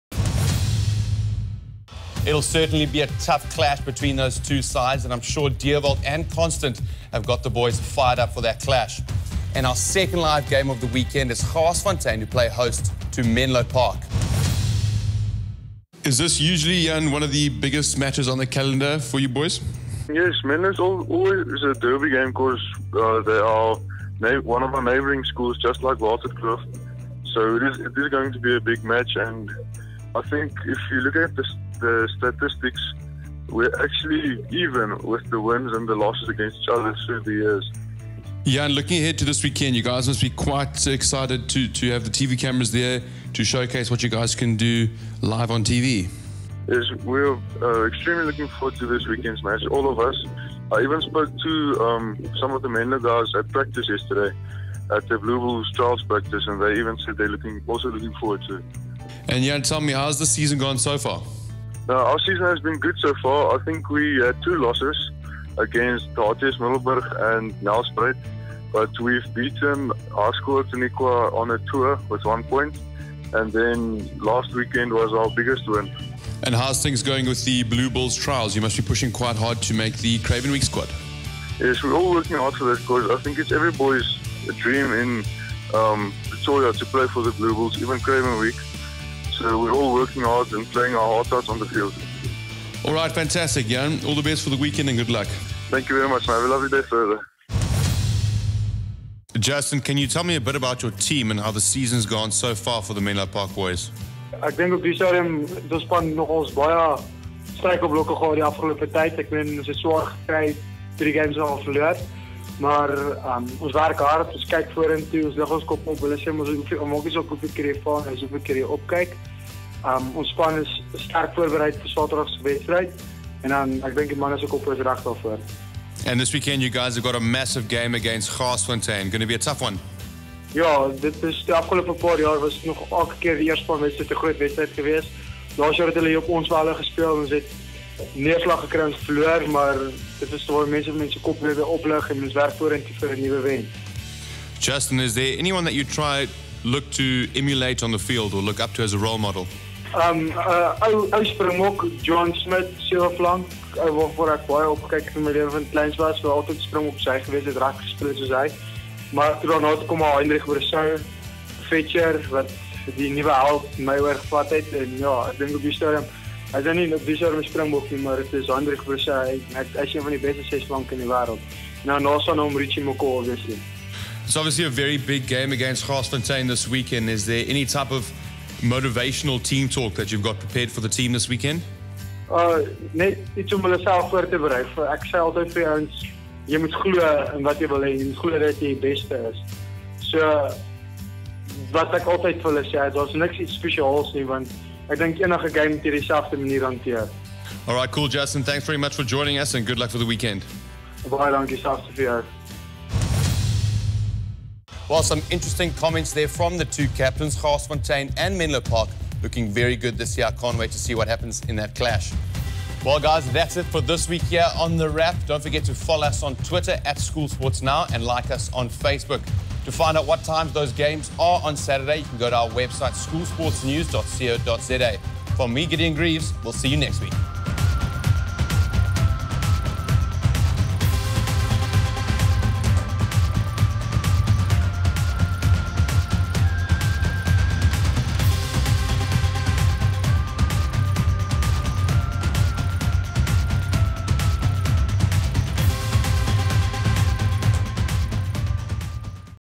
Interviews: Jan-Henning Campher & Justin Meintjies
Pre-match interviews leading up to the game between Hoërskool Menlopark and Hoërskool Garsfontein.